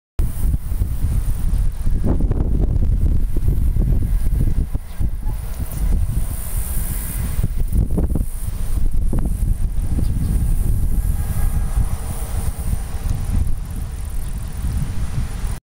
Small BNSF yard
Small BNSF yard in North Little Rock Arkansas on September 1 2014  with Little Rock Port Authority locos